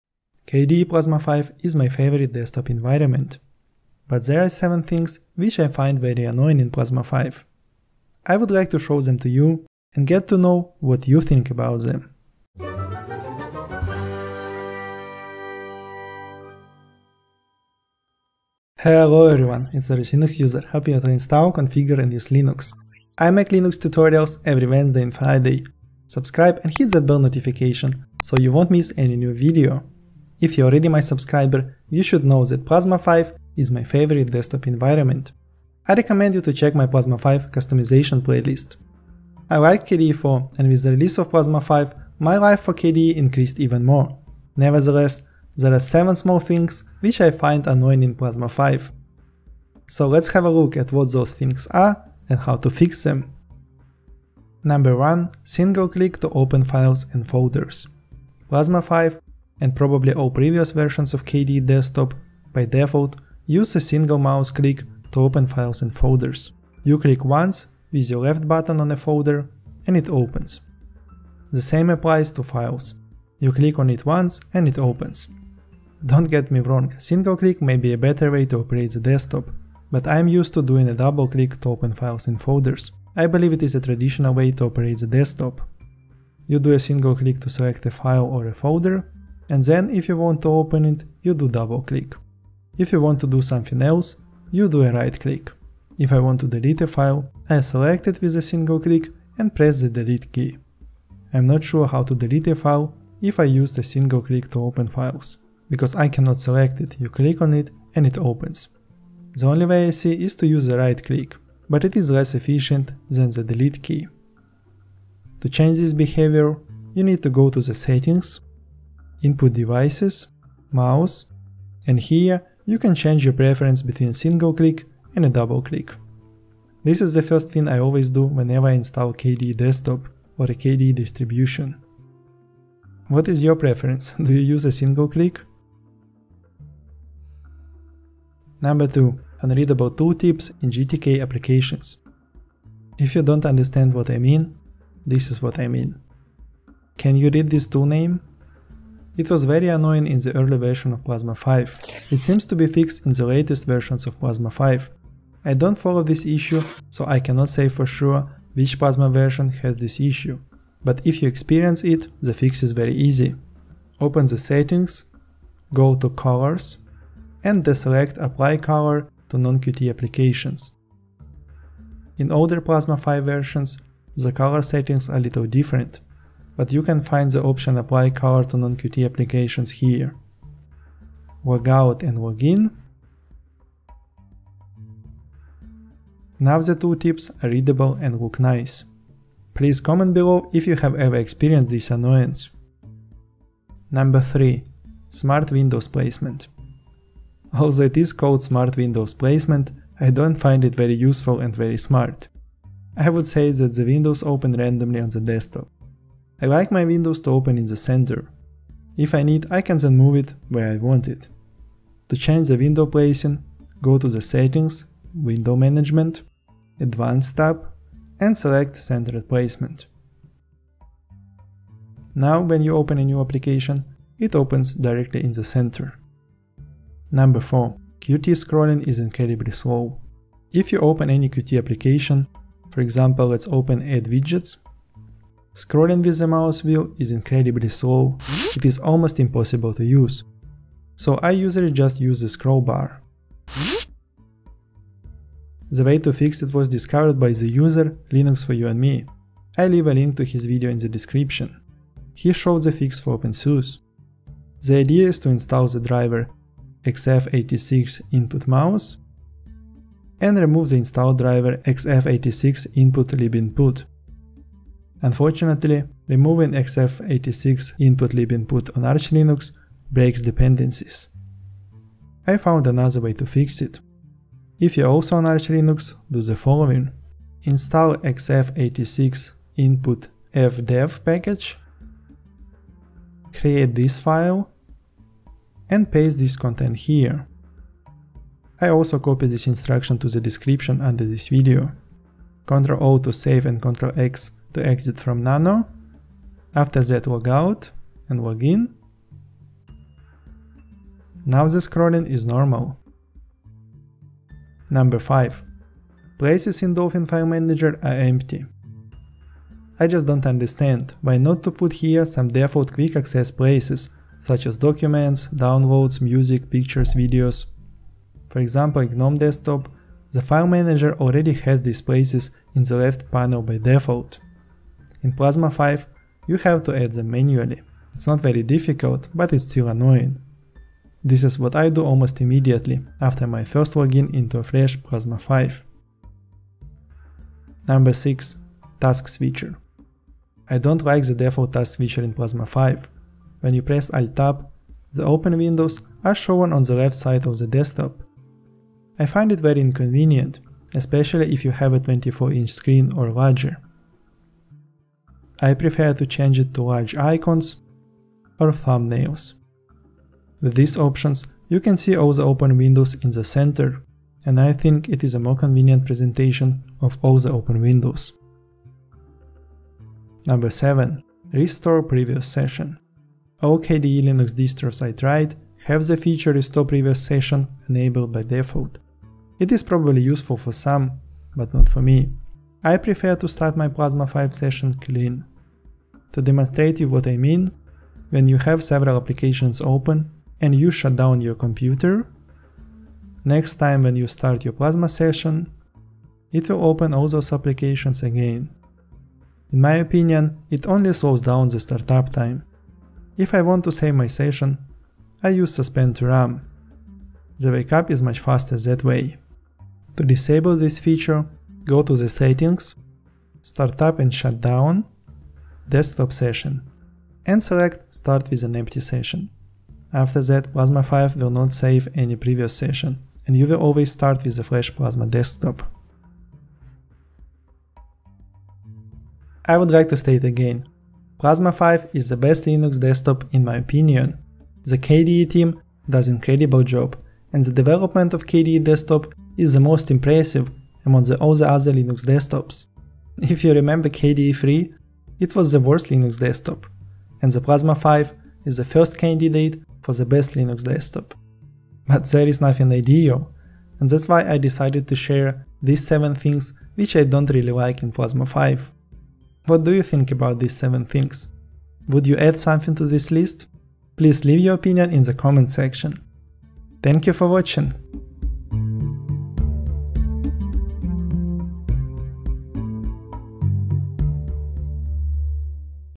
7 annoyances of Plasma 5 KDE
KDE Plasma 5 is my favorite desktop environment. But there are 7 things which I find very annoying in Plasma 5. I would like to show them to you and get to know what you think about them.

0:54 Single-click to open files and folders
2:18 Unreadable Tooltips in GTK applications
3:13 Smart windows placement.
3:47 Qt scrolling is incredibly slow
5:04 Places in Dolphin file manager are empty
5:37 Task Switcher
6:10 Restore Previous Session.

The way to fix Qt scrolling was discovered by Linux4UnMe He showed the fix for OpenSuse.

To fix it on Arch and its derivatives:
Create this file /etc/X11/xorg.conf.d/99-user.conf
And paste this content there:

Section "InputClass"
        Identifier "evdev pointer catchall"
        MatchIsPointer "on"
        MatchDevicePath "/dev/input/event*"
        Driver "evdev"
EndSection